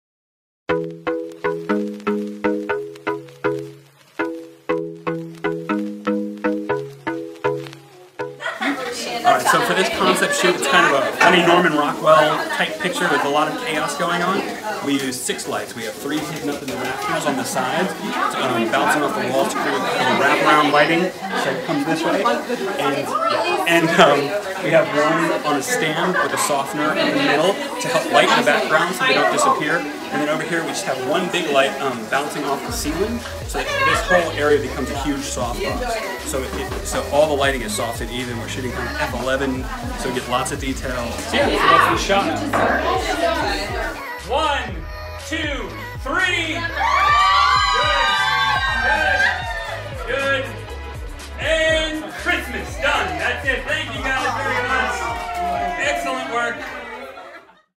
Team Image Crazy Kitchen BTS
Quick Demo of our shoot of large group in a kitchen, making a mess.  the concept and lighting

us checkout Bohemianphotography.com